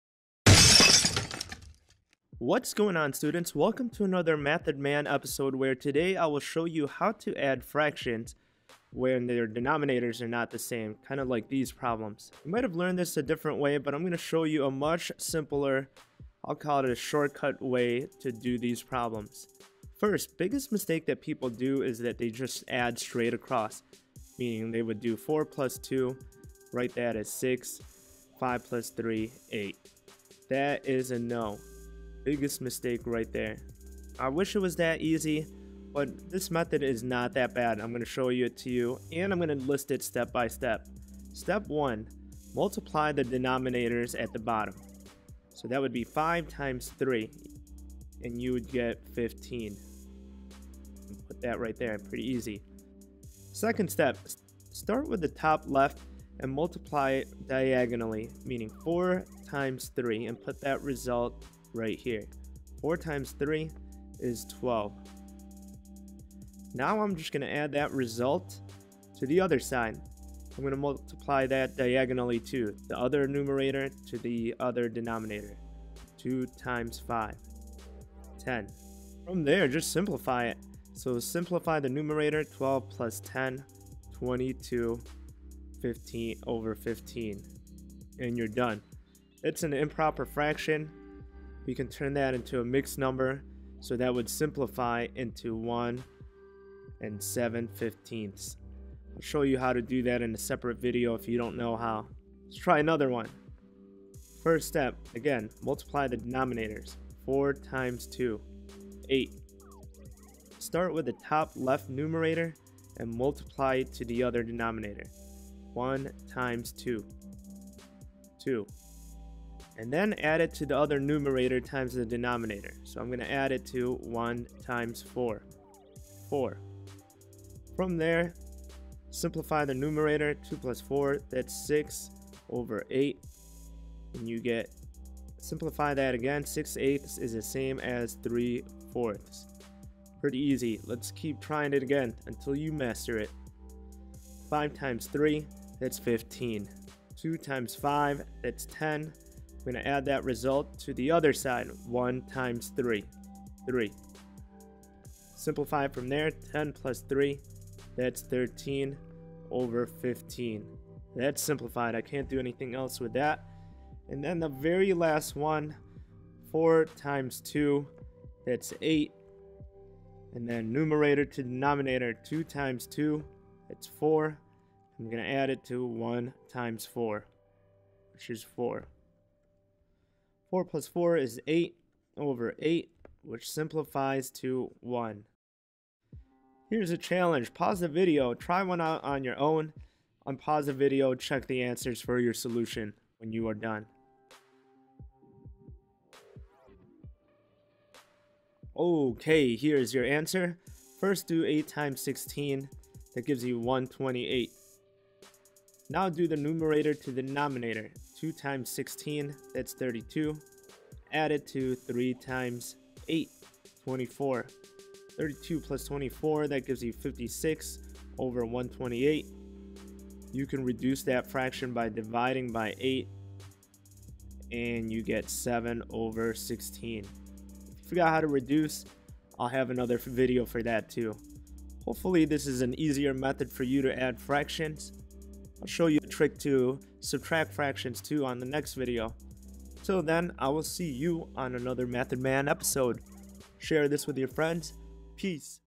Add Fractions With Different Denominators Shortcut
This video is a shortcut to adding fractions with unlike denominators.  After this video, it will become so easy that you can do it in your head (no calculator).  The steps are really easy and I will list them down below.

Steps:
1)  Multiply the denominator of both fractions.   That result will the the answer's denominator.

Cross Multiplication Process
2)  Multiply the 1st numerator to the 2nd denominator.
3)  Multiply the 2nd numerator to the 1st denominator.
4)  Add your result from 2 and 3 and write the result to the answer's numerator

5)  Simplify.

Sounds really confusing, then watch the video and you will soon add fractions in your head = )